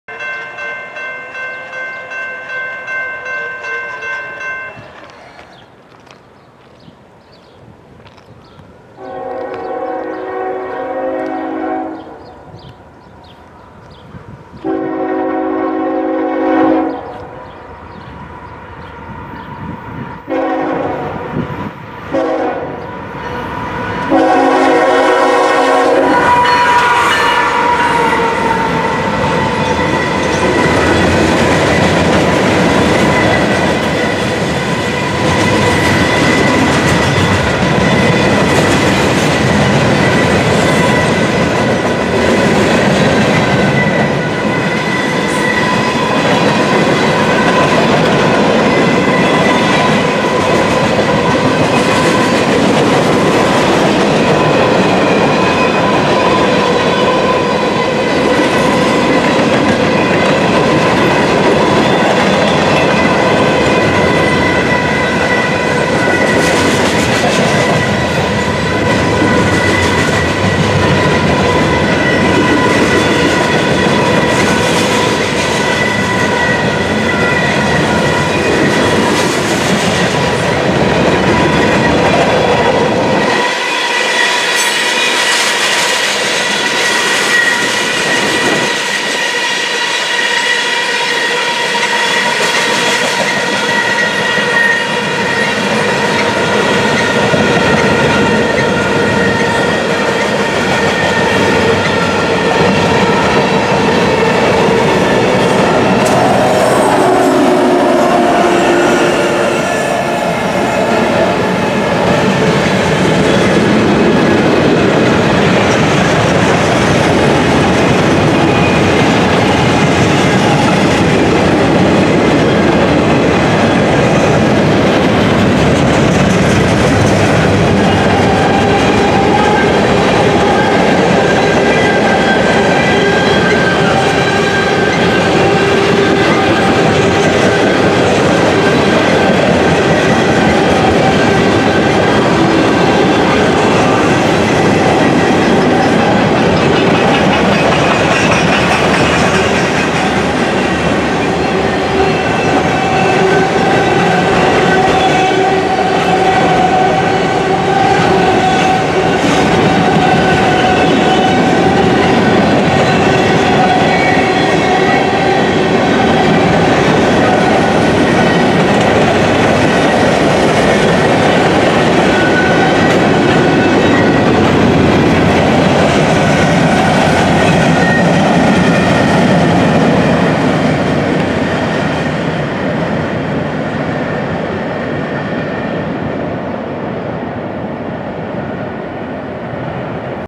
NORFOLK SOUTHERN GE ES44AC Northbound Manifest Mix Freight
A northbound Mix Freight Lead by Norfolk Southern GE ES44AC #8039 Engine passing through at Milepost Rosemont.